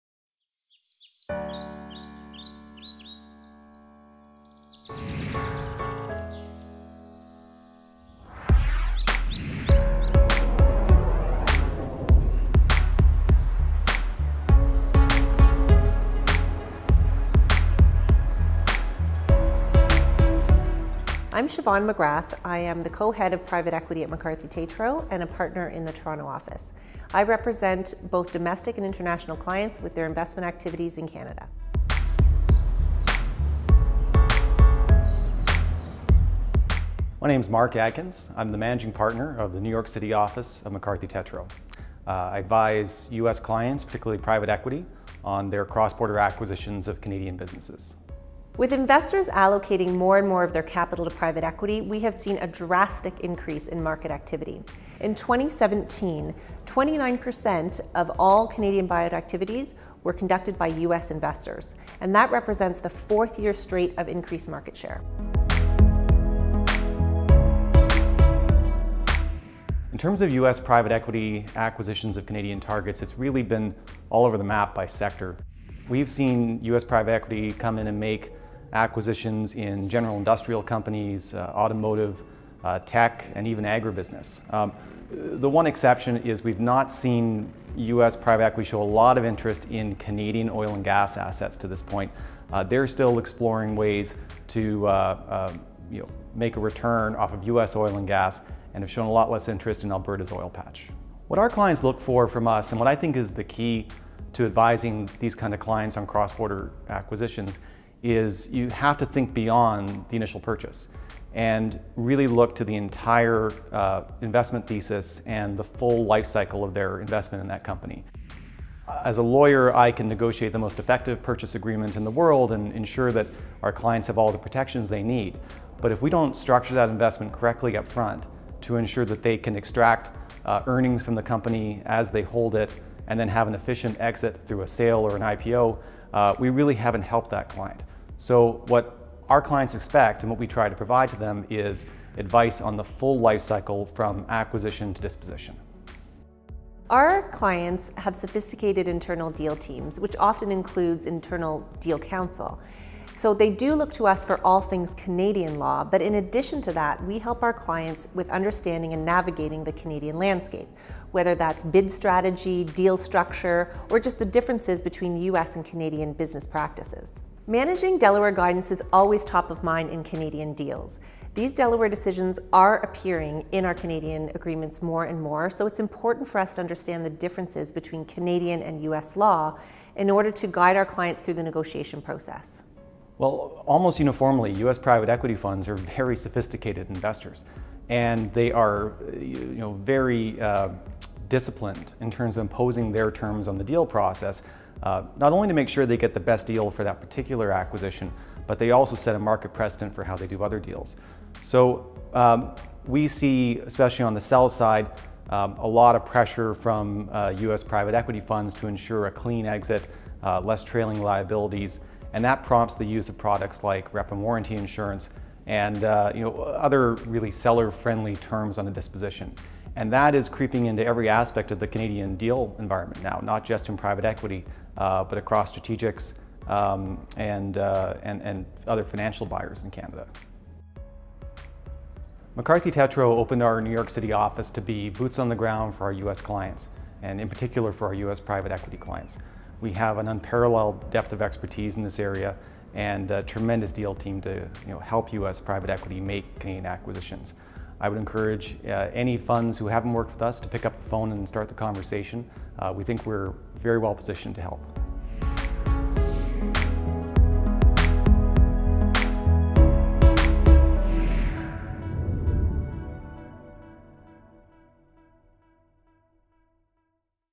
Helping U.S. Clients Cross Borders
In addition to advising on Canadian law, McCarthy Tétrault’s Private Equity Team plays the role of translator, guiding our clients through the differences between the U.S. and Canadian markets. The latest edition in McCarthy Tétrault’s Private Equity video series features NYC Managing Partner Mark Adkins and Shevaun McGrath, Co-head of our Private Equity Group, describing the intricacies U.S. investors face with their inbound Canadian legal matters.